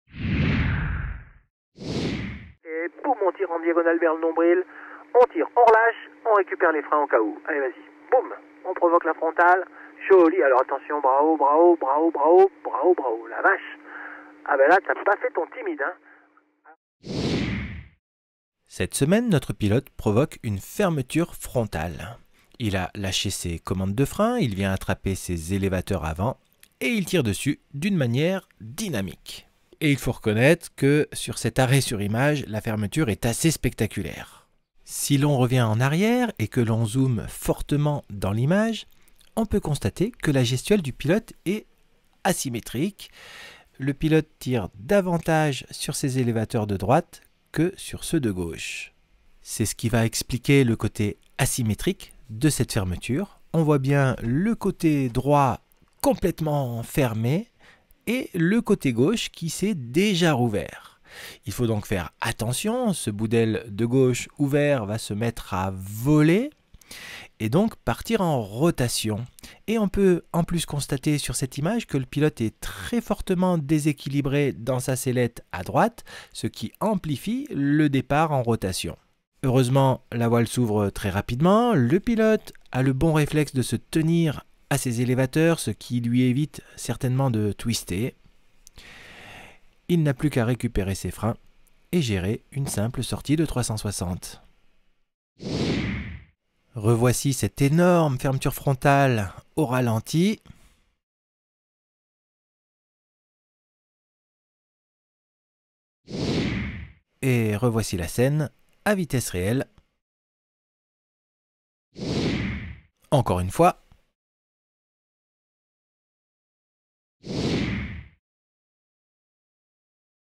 Fermeture frontale asymétrique!
Un stage de pilotage est l’occasion de provoquer toutes sortes de fermetures afin de les démystifier et de connaître sa voile.

Une grosse fermeture frontale donne souvent des images assez spectaculaires. Si la fermeture est asymétrique, la voile peut prendre des formes très impressionnantes!

Bien souvent on est surpris de voir que les parapentes peuvent se regonfler très rapidement… Si le pilote ne commet pas d’erreurs, et s’il ne twiste pas, on revient vite à une situation sous contrôle…